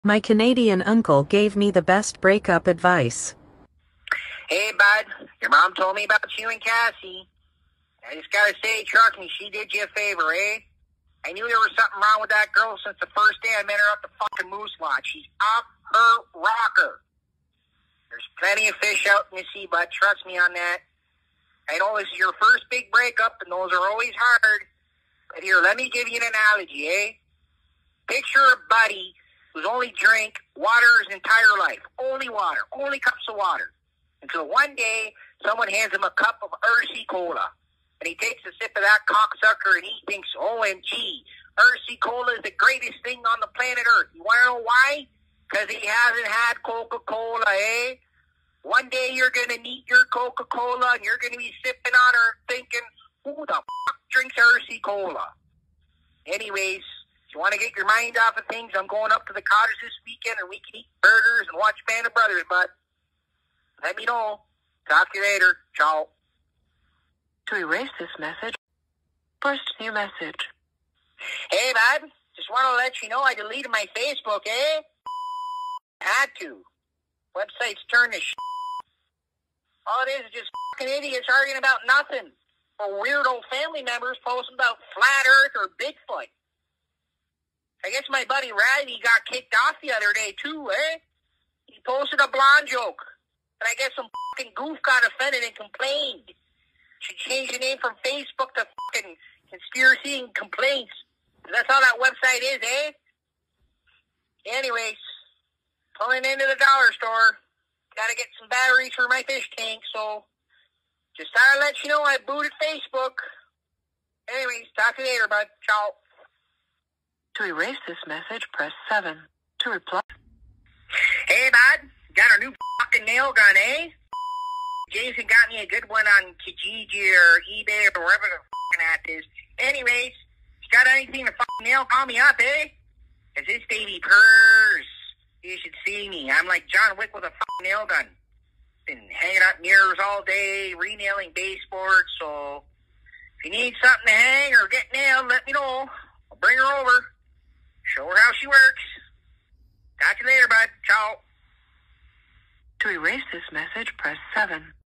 Canadian Uncle Voicemail Compilation
Here is a compilation of my very Canadian Uncle's Voicemail he leaves me Featuring some great break up advice.